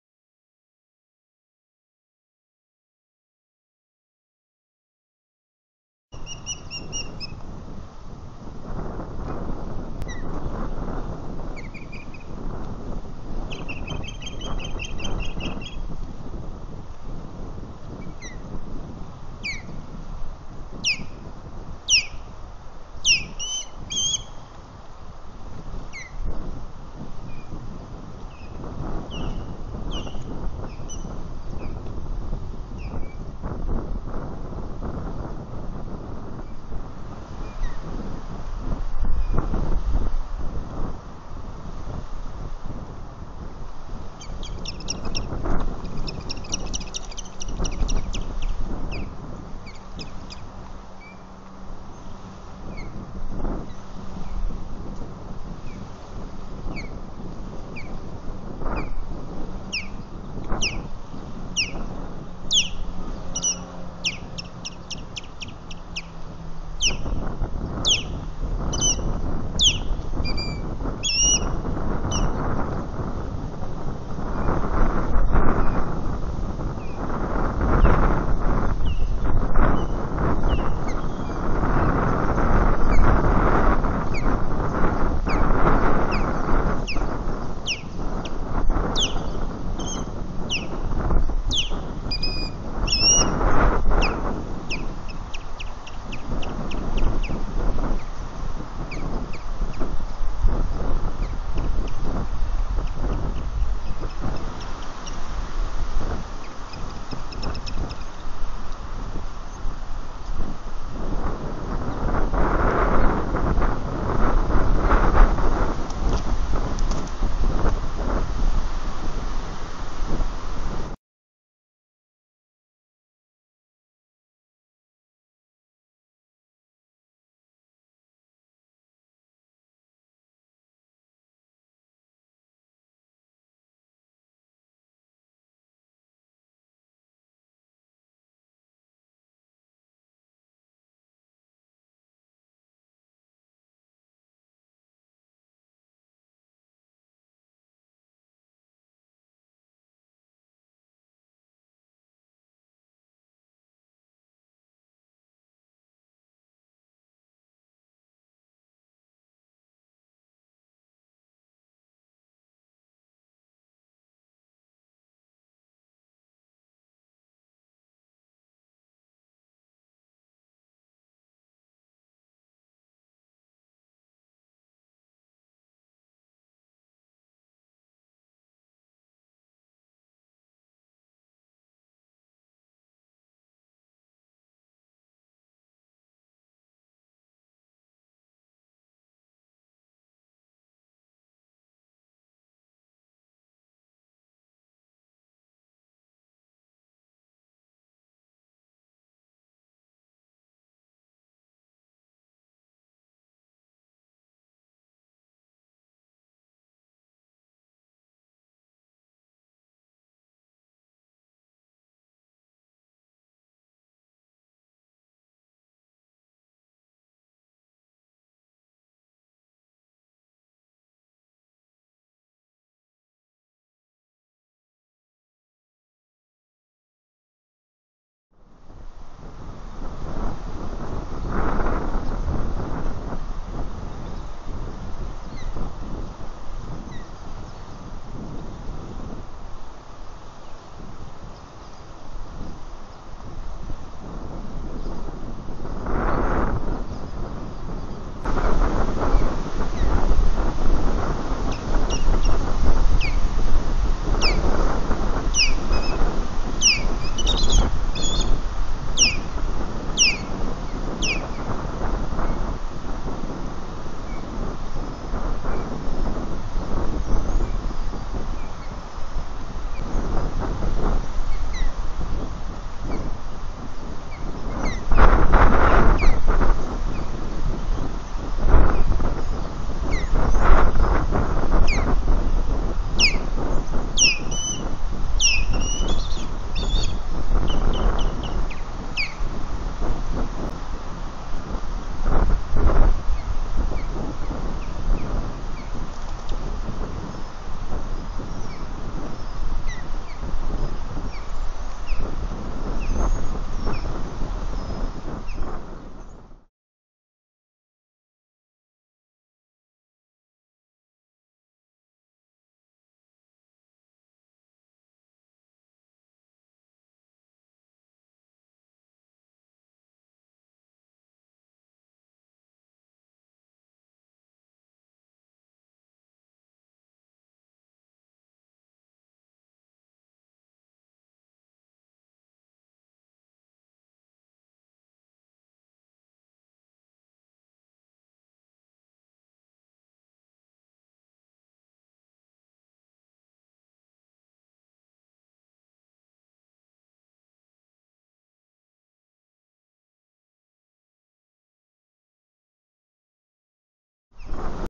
Lake Murray Osprey Lucy says this is my nest 2:20pm 3-3-2020
Lucy came to the nest alerting. O's flying overhead. When Duncan comes to the nest there is a 3rd O following him. This O circles around and comes back landing on the side of the railing. Lucy is not happy and chases away what looked like a Female to me maybe the one that had been on the nest before Lucy came home I am not sure. At 2:35pm Lucy came back to the nest again later and again an O tried to land but she wouldn't let it. This time it was hard to tell who it was.